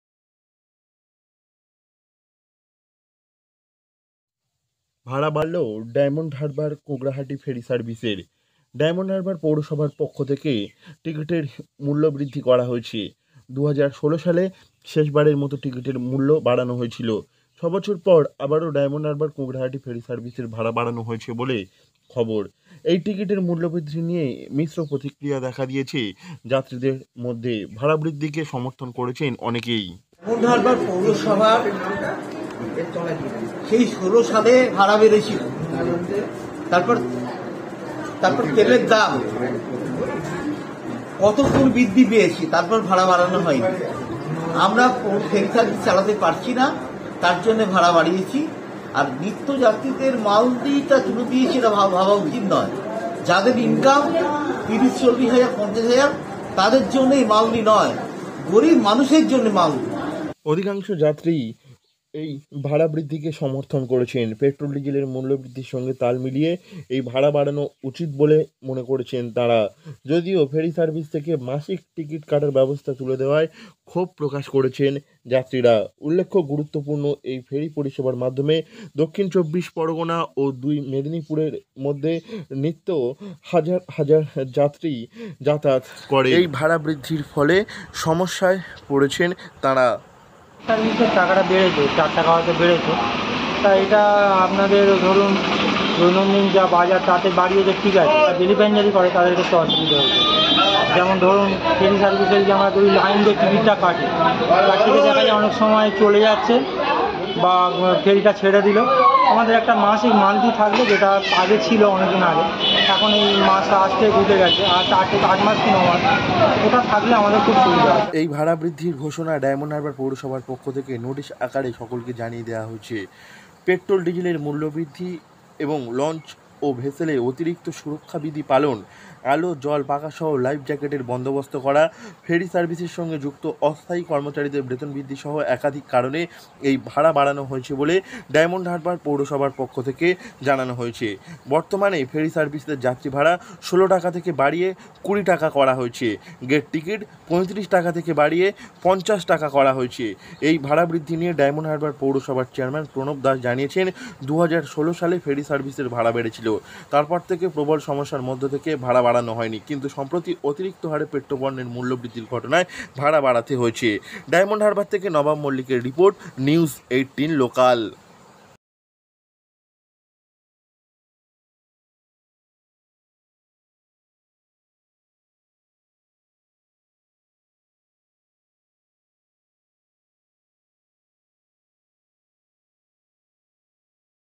ভাড়া বাড়ল ডায়মন্ডহারবার-কুঁকরাহাটী ফ
ভাড়া বাড়ল ডায়মন্ডহারবার-কুঁকরাহাটী ফেরী সার্ভিসের। যাত্রীভাড়া ১৬ টাকা থেকে বাড়িয়ে করা হয়েছে ২০ টাকা।